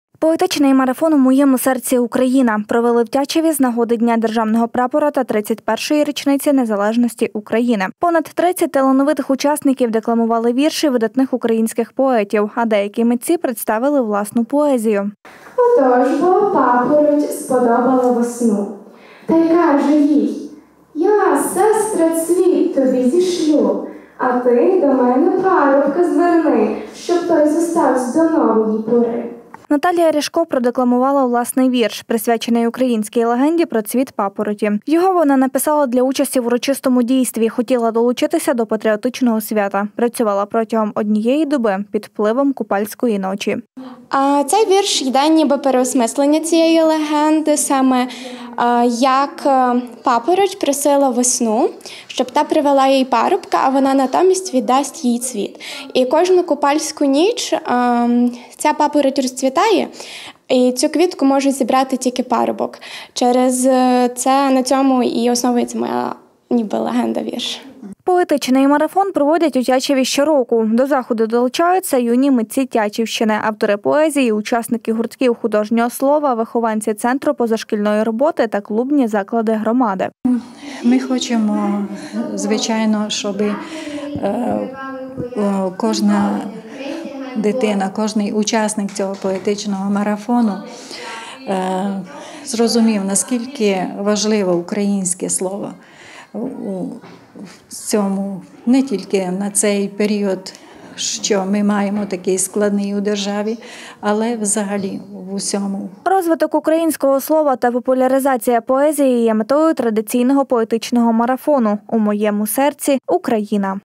Поетичний марафон
Традиційне мистецько-патріотичне дійство відбулося в Тячеві – щороку талановиті діти беруть участь в поетичному марафоні «В моєму серці – Україна». Захід приурочений до Дня Державного Прапора України та 31-ї річниці Незалежності. Цьогоріч долучилося понад 30 учасників. Мета святкового заходу – популяризація українського слова та поезії. Юні учасники декламували вірші видатних українських поетів, а деякі митці представили власні твори. Поетичний марафон «В моєму серці – Україна» провели в Тячеві з нагоди Дня Державного Прапора України та 31-ї річниці Незалежності. Понад 30 талановитих учасників декламували вірші видатних українських поетів, а деякі митці представили власну поезію. Вірш  Наталія Рішко продекламувала власний вірш, присвячений українській легенді про цвіт папороті. Його вона написала для участі в урочистому дійстві – хотіла долучитися до патріотичного свята. Працювала протягом однієї доби – під впливом Купальської ночі.  Поетичний марафон проводять в Тячеві щороку – до заходу долучаються юні митці Тячівщини – автори поезії, учасники гуртків художнього слова, вихованці центру позашкільної роботи та клубні заклади громади.  Розвиток українського слова та популяризація поезії є метою традиційного поетичного марафону « В моєму серці – Україна».